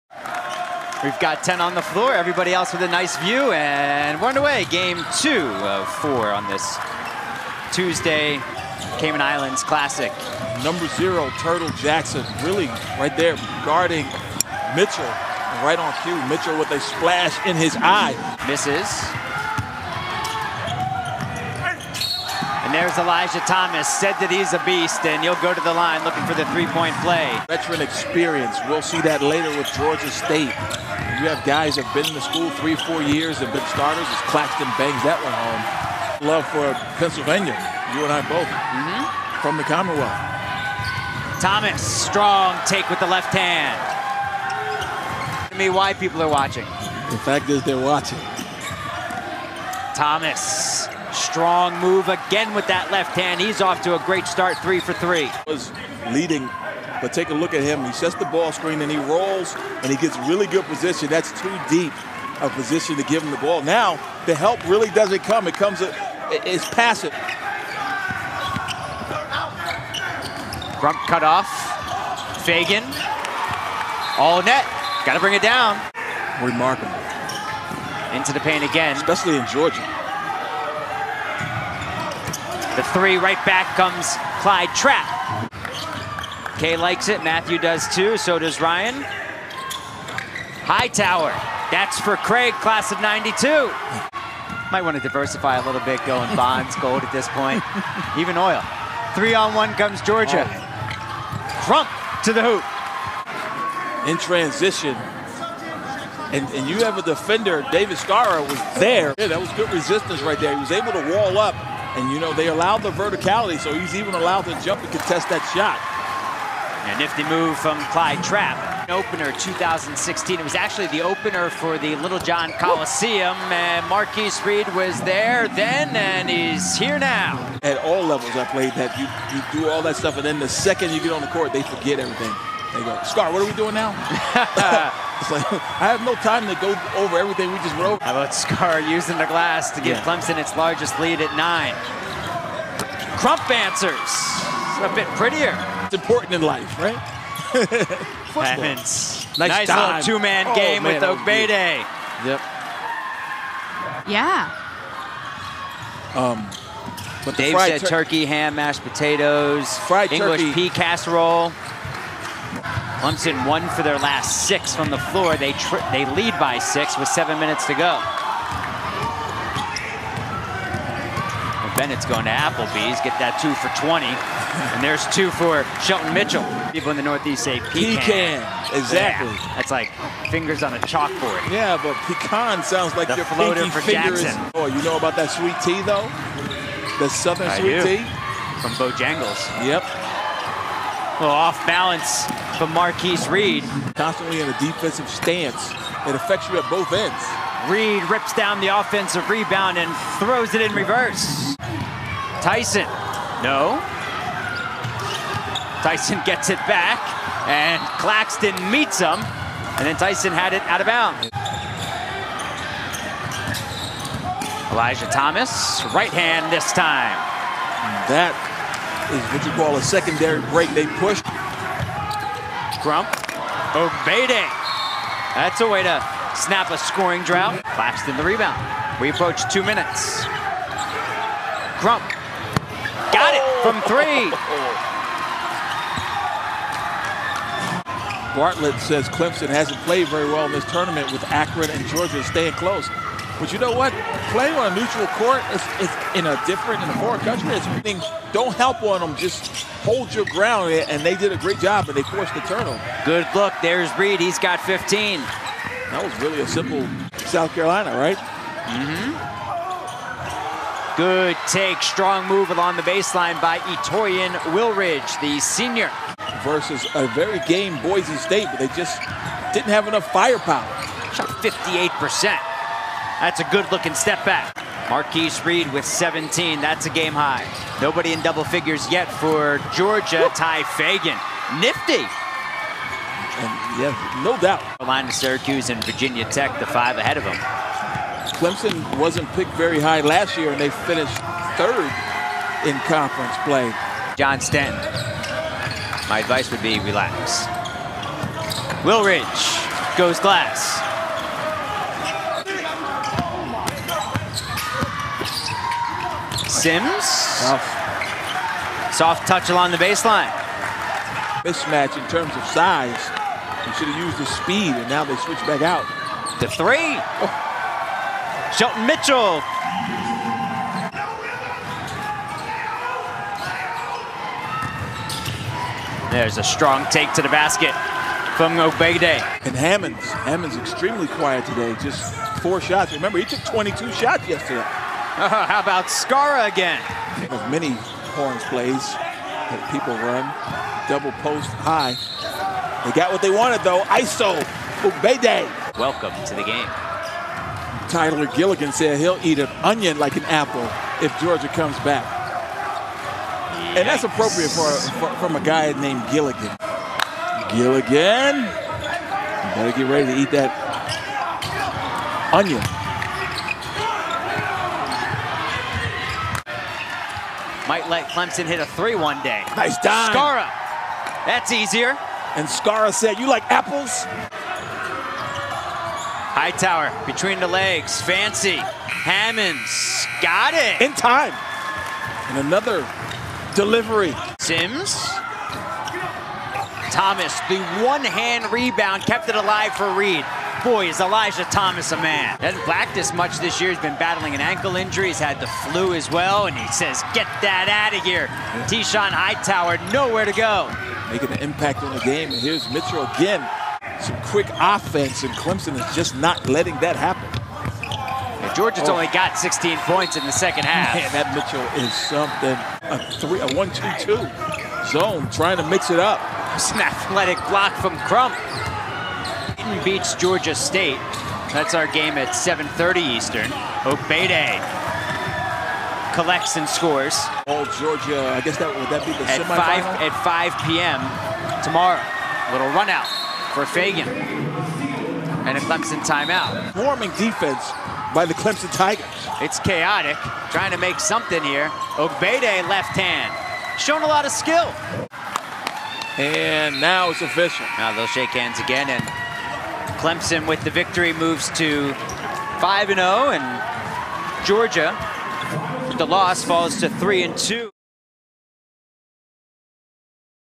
Clemson vs. Georgia Condensed Game | 2018-19 ACC Basketball
Watch the Condensed Game from Georgia Bulldogs vs. Clemson Tigers, 11/20/2018